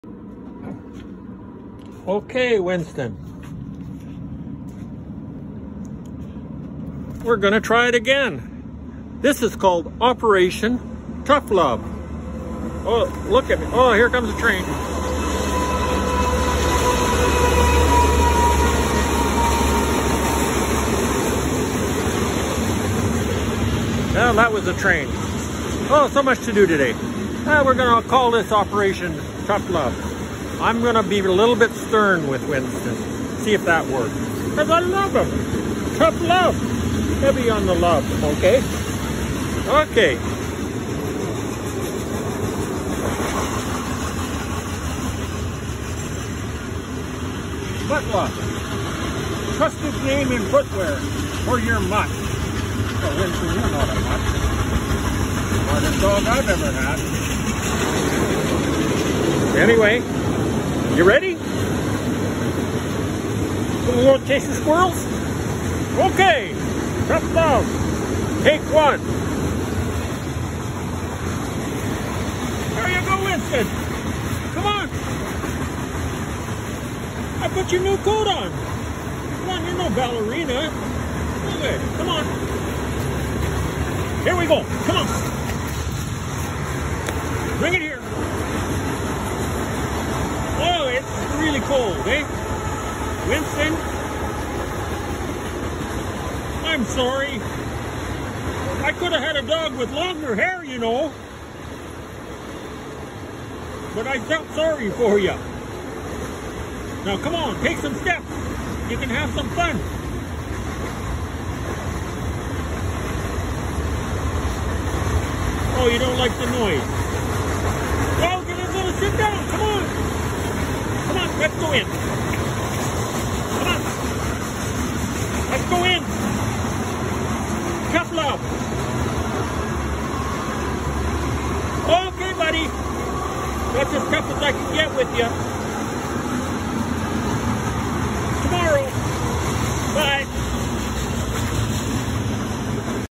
My little Boston Terrier Chihuahua
Trying to get my dog to use his sub zero bootys
Thinking to try Tough ❤️